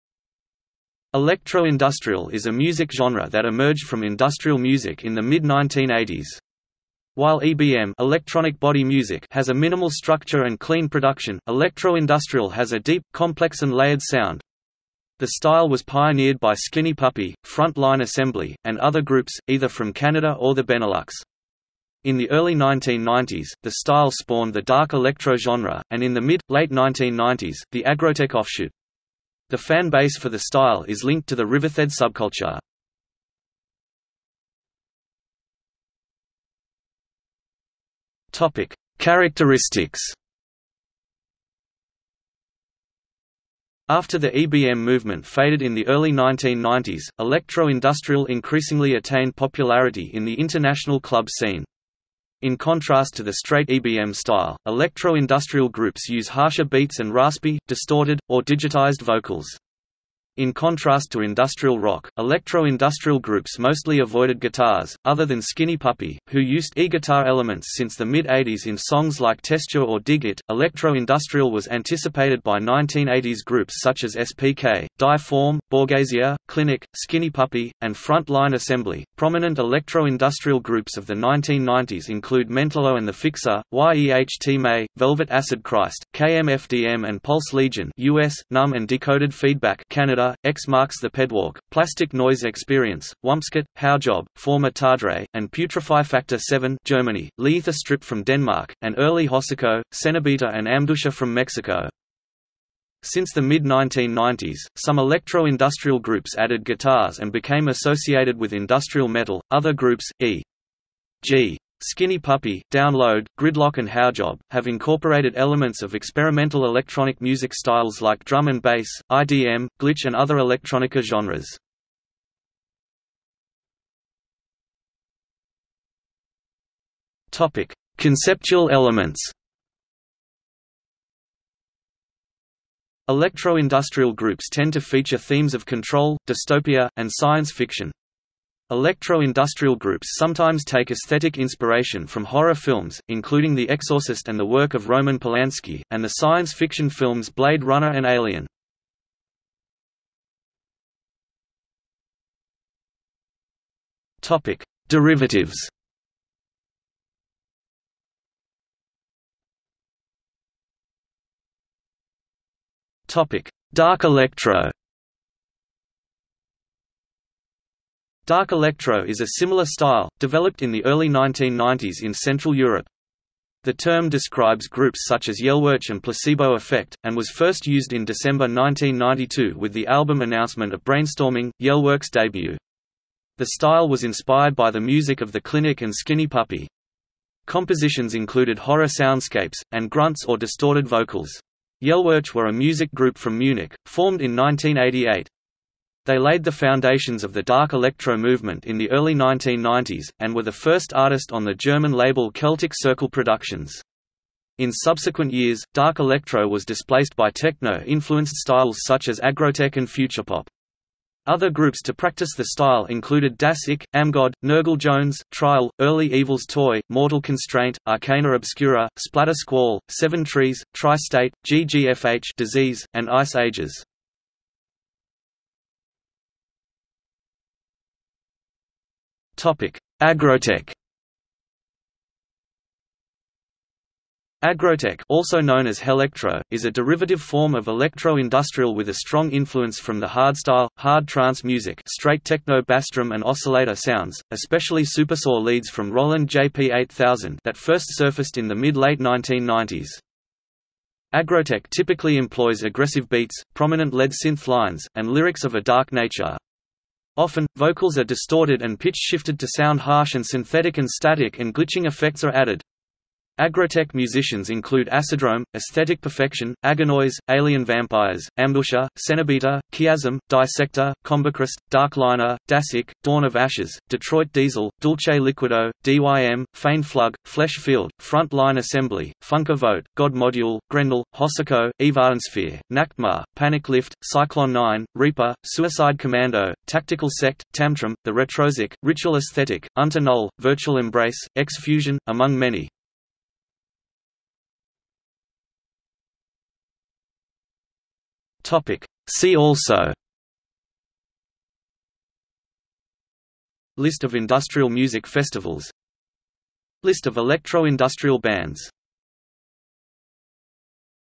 Electro-industrial | Wikipedia audio article
00:00:34 1 Characteristics
00:02:03 2 Conceptual elements
00:02:30 3 Derivatives
00:02:39 3.1 Dark electro
00:03:45 3.2 Aggrotech
00:04:54 4 See also

- Socrates

SUMMARY
Electro-industrial is a music genre that emerged from industrial music in the mid-1980s. While EBM (electronic body music) has a minimal structure and clean production, electro-industrial has a deep, complex and layered sound. The style was pioneered by Skinny Puppy, Front Line Assembly, and other groups, either from Canada or the Benelux. In the early 1990s, the style spawned the dark electro genre, and in the mid-/late-1990s, the aggrotech offshoot. The fan base for the style is linked to the rivethead subculture.